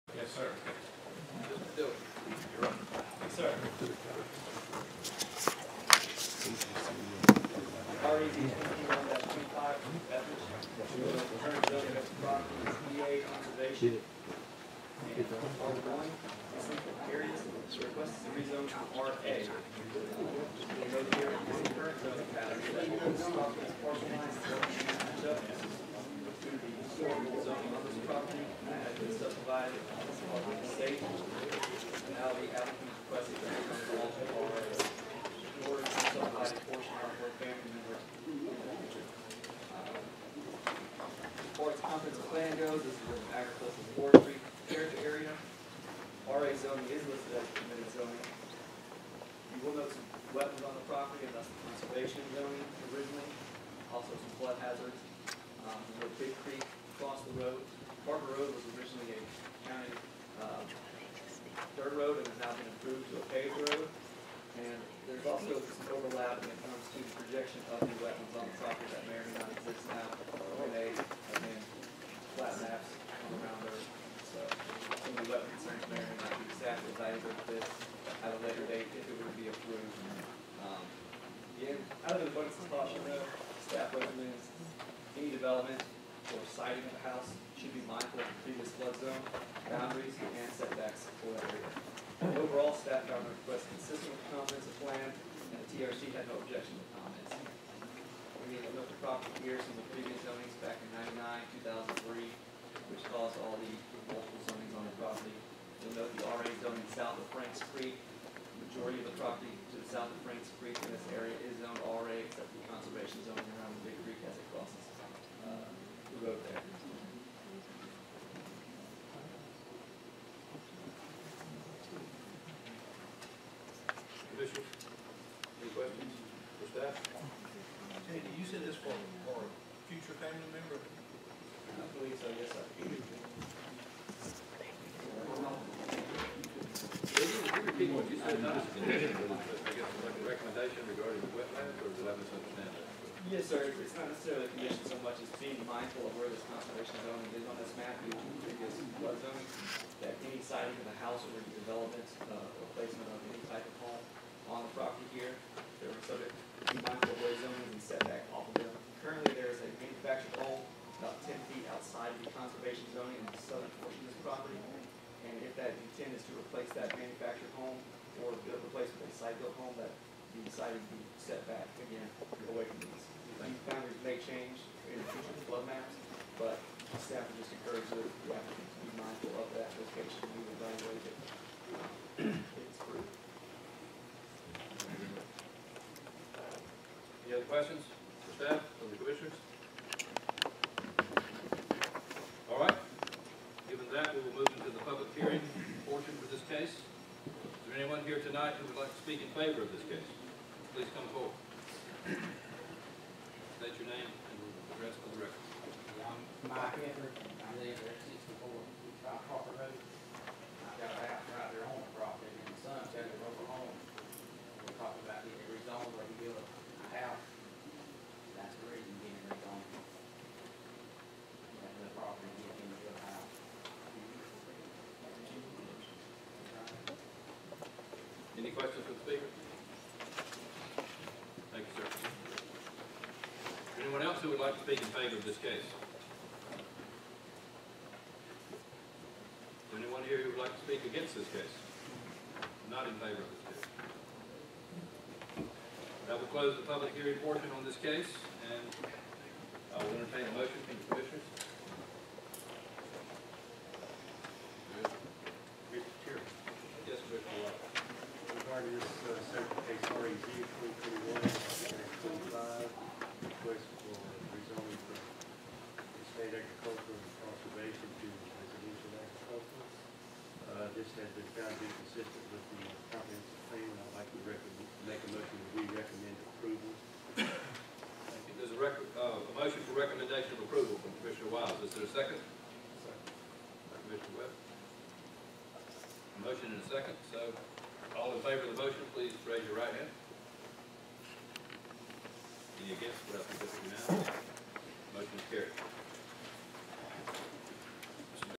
7. REZ 2021-25 Etheridge, Parker Rd 5.6ac EA to RA @ GLPC 2021-11-29
Greater Lowndes Planning Commission - Regular Session - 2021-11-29

This is for a family division.

Approved unanimously.